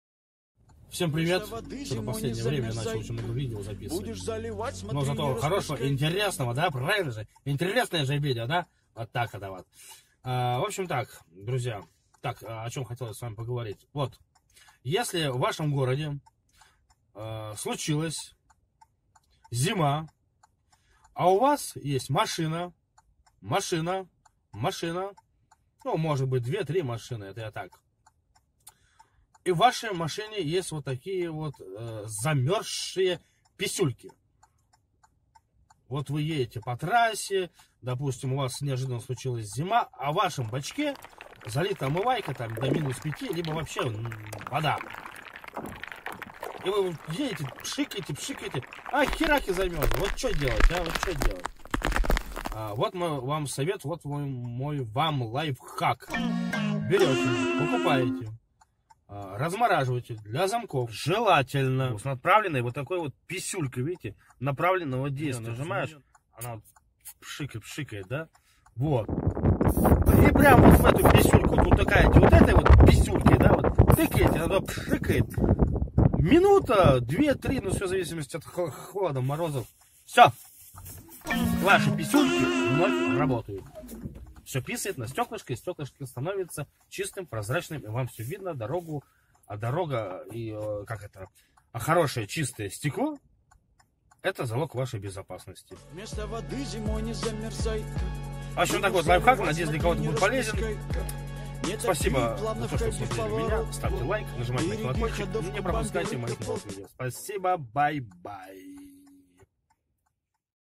Что делать если замёрзла Форсунка Омывателя
В этом видео я вам расскажу, что делать если у вас замёрзли форсунки омывателя лобового стекла. Идея проверенная лично мной!!!